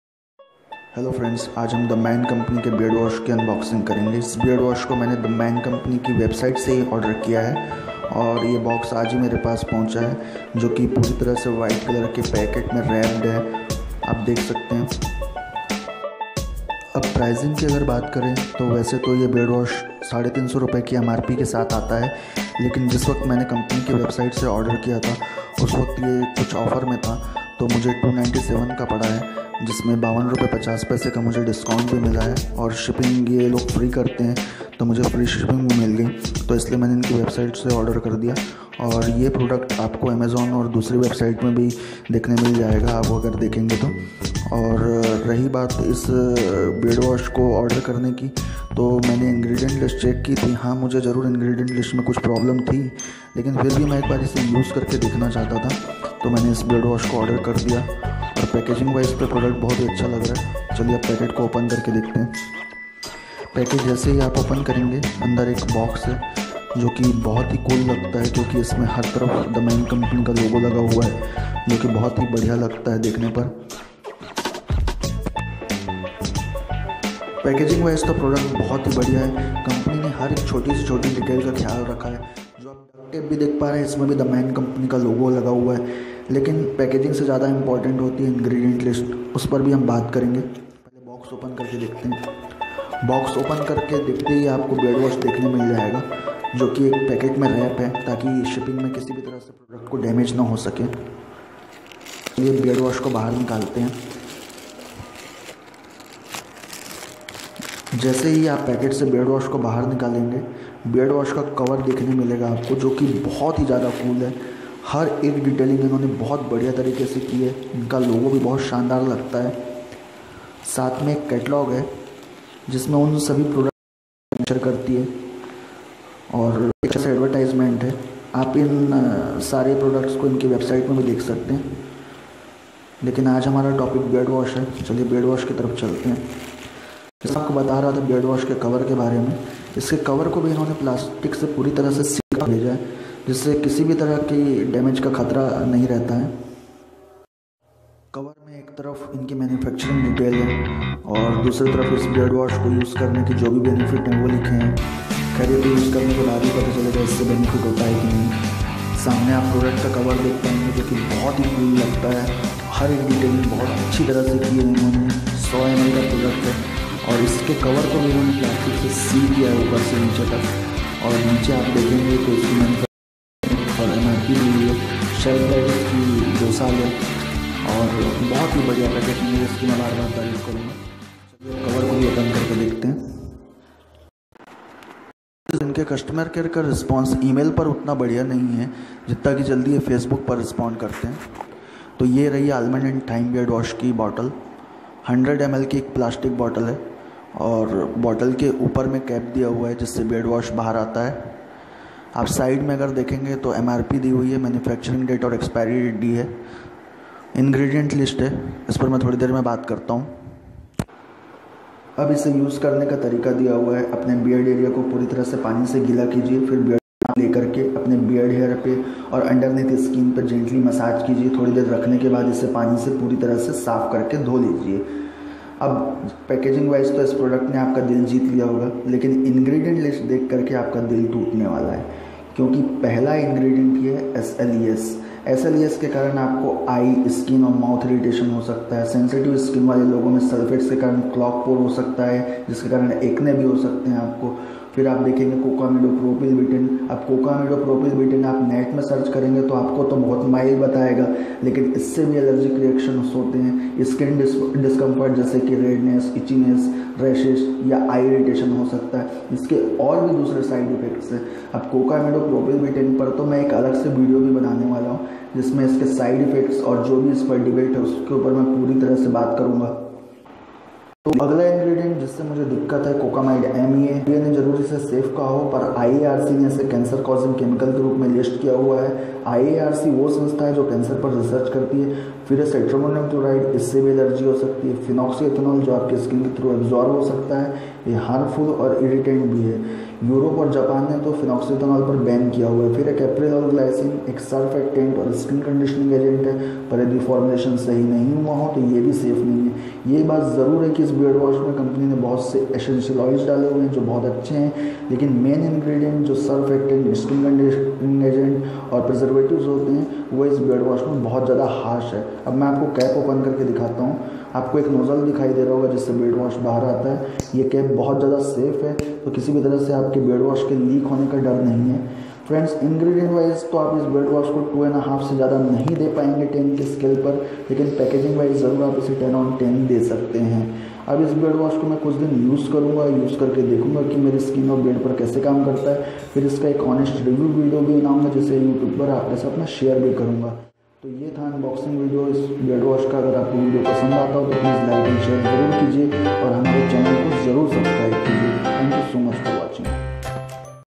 The Man Company Beard Wash Unboxing | Almond & Thyme Beard Wash Ingredients Details
Honest review of the man company beard wash
Unboxing video of the man company beard wash
Detail ingredient's review